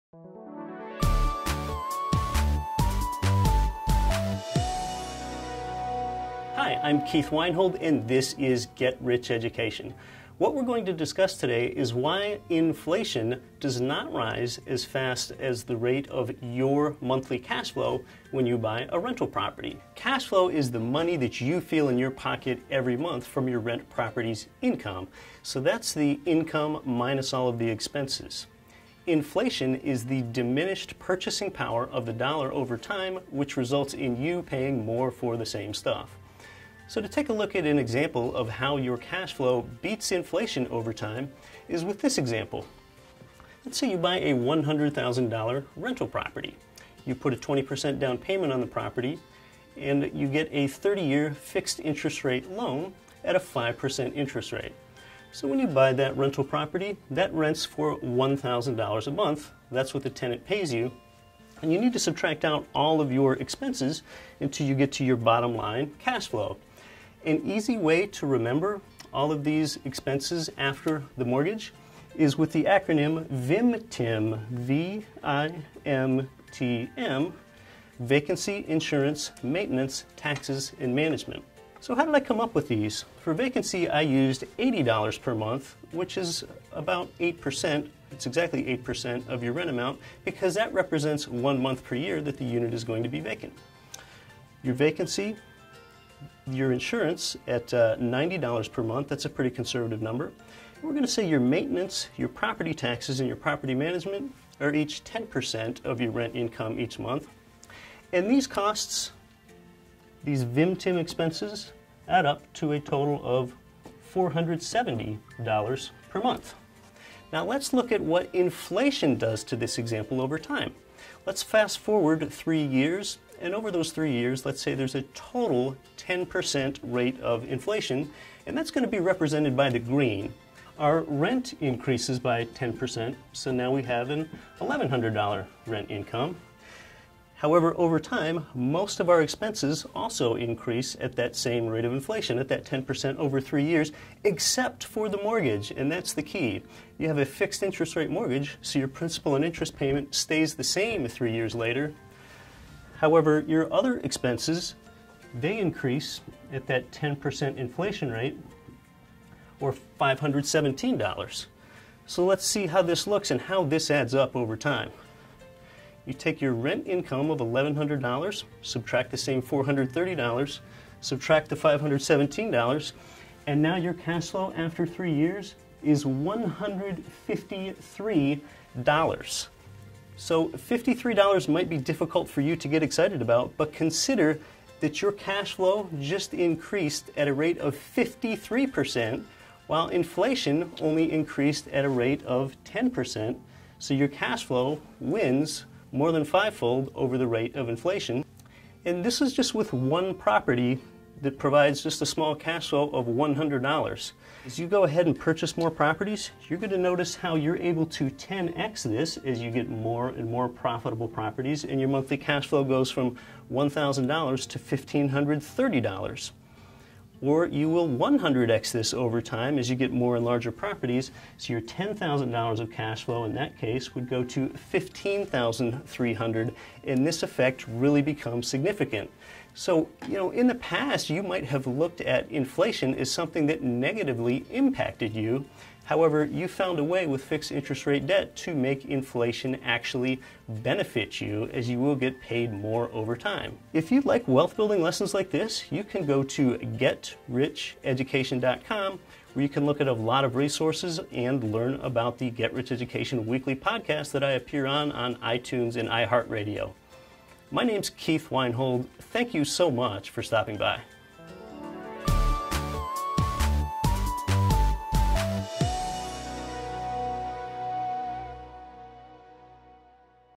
Get Rich Education - Cash Flow vs. Inflation
Listen to all of our shows with the Get Rich Education listener app! Apple | Android.

Real estate investing creates wealth five ways at the same time! The least known of these five is how you actually profit from inflation.